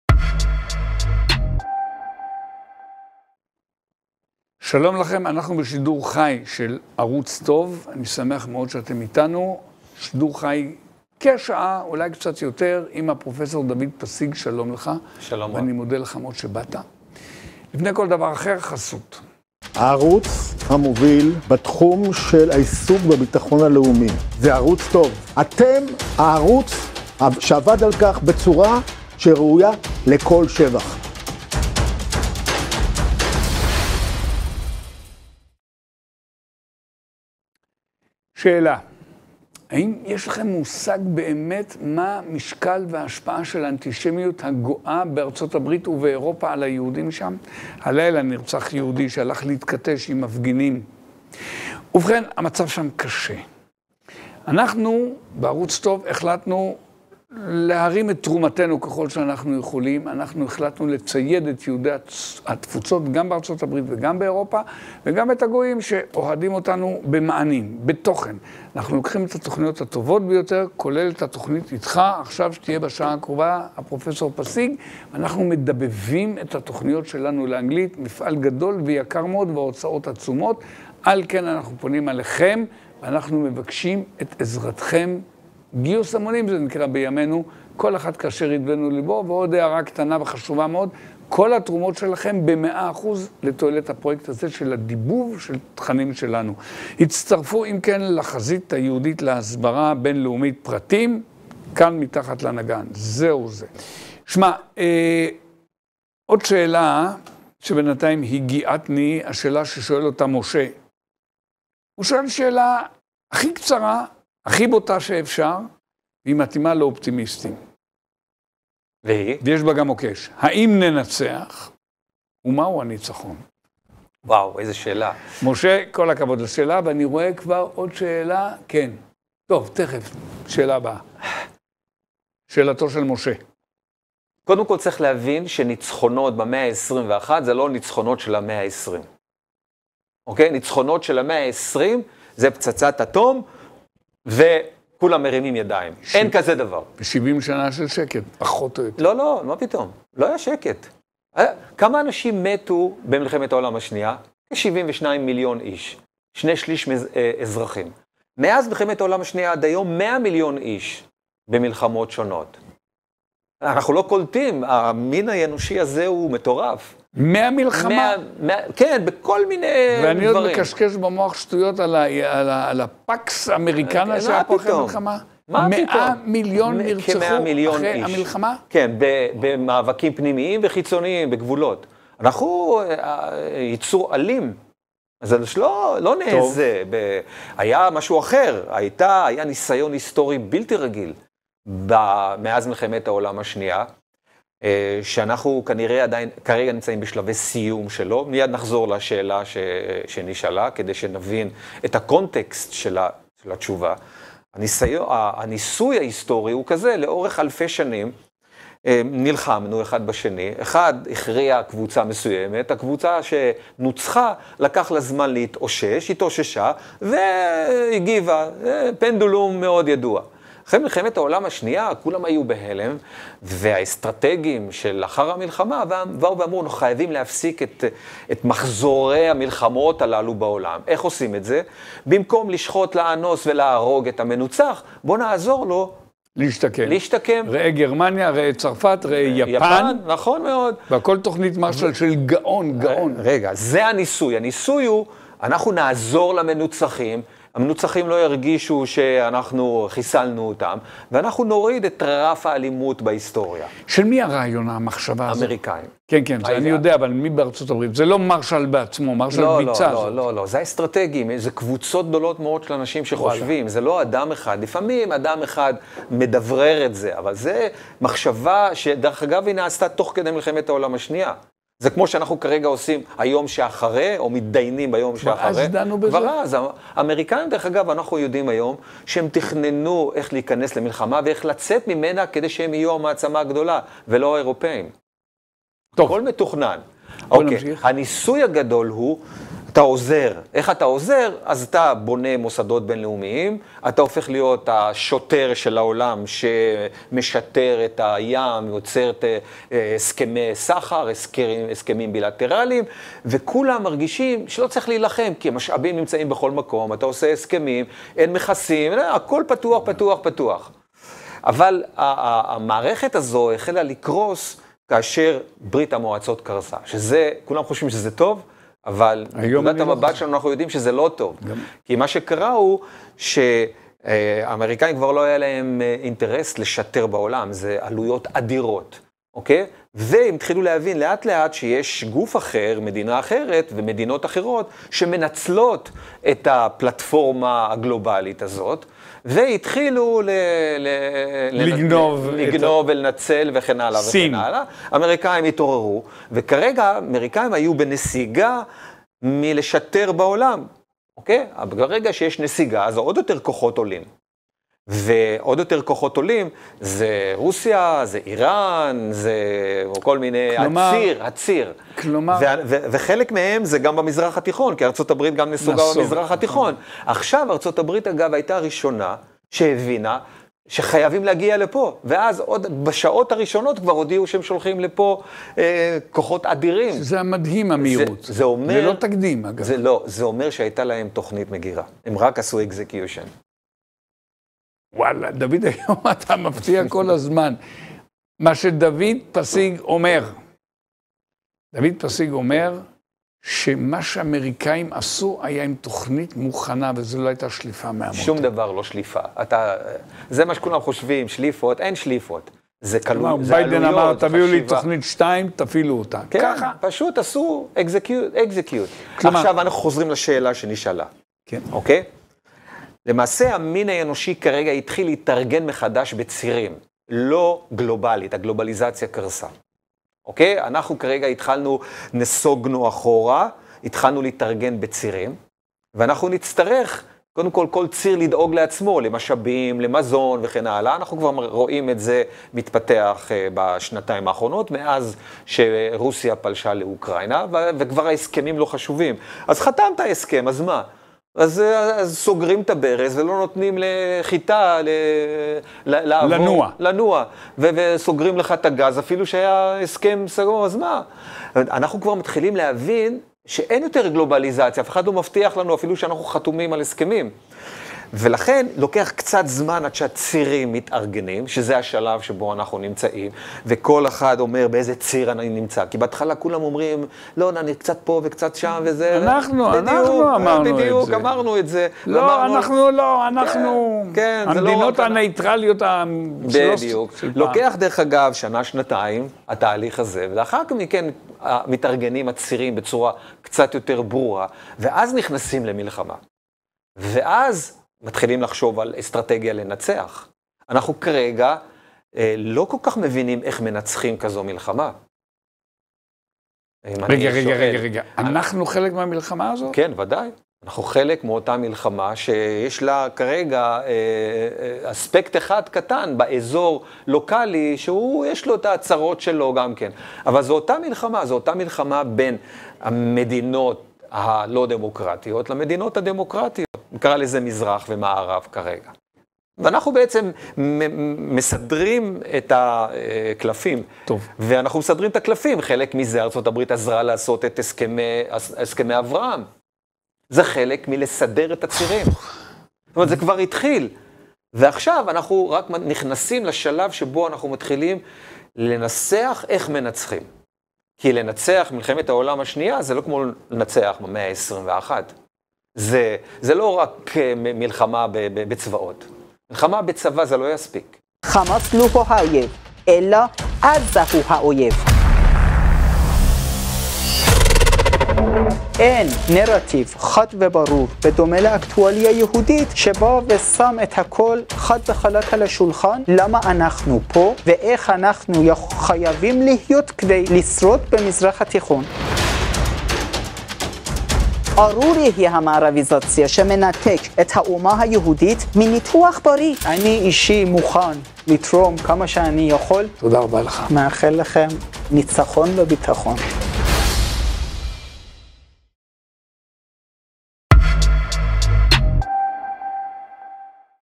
העתידן פרופ' דוד פסיג עונה לשאלה הגדולה: האם ננצח במלחמה?
*לתמיכה בהסברה יהודית בחזית הבינלאומית:*

"ישראל נלחמת","חרבות ברזל | כרגע אנחנו בשלב של התארגנות מחדש של הצירים"
"ישראל נלחמת","חרבות ברזל | כרגע לא ברור לנו איך מנצחים כזאת מלחמה"

4640 אלעזר שטרום+פרופ' דוד פסיג האם ננצח במלחמה